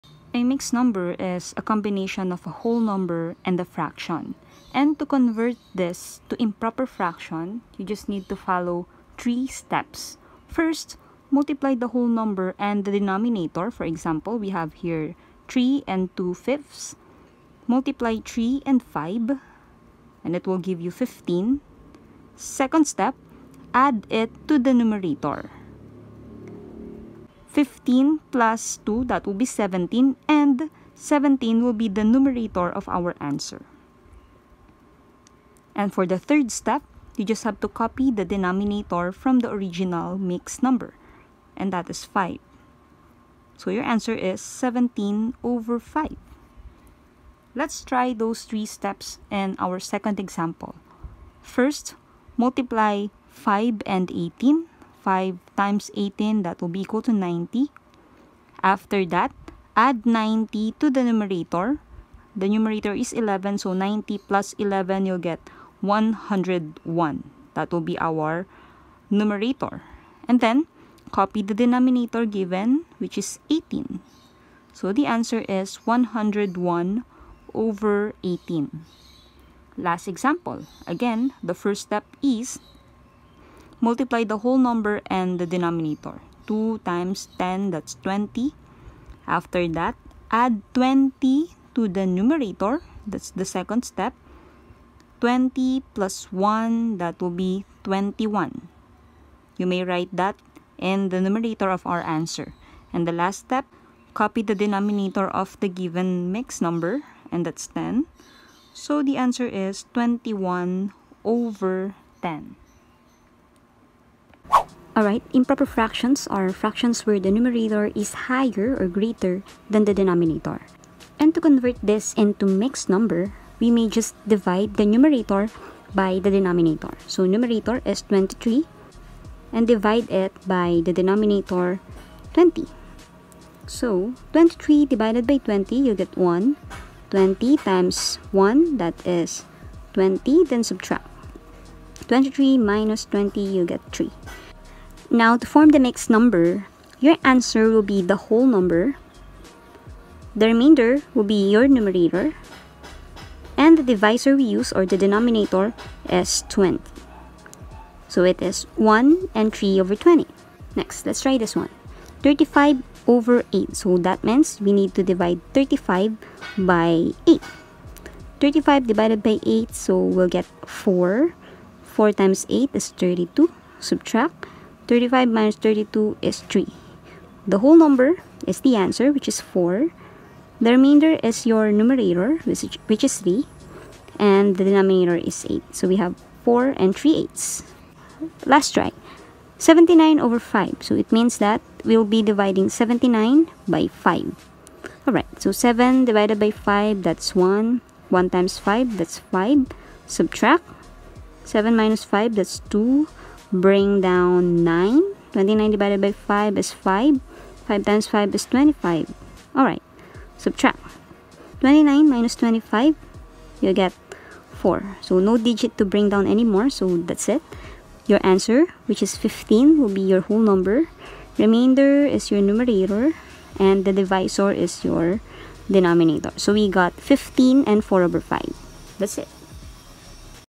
Converting a Mixed Number to Improper Fraction and vice versa | Easiest Way
This video is about how to convert mixed number to improper fraction and improper to mixed.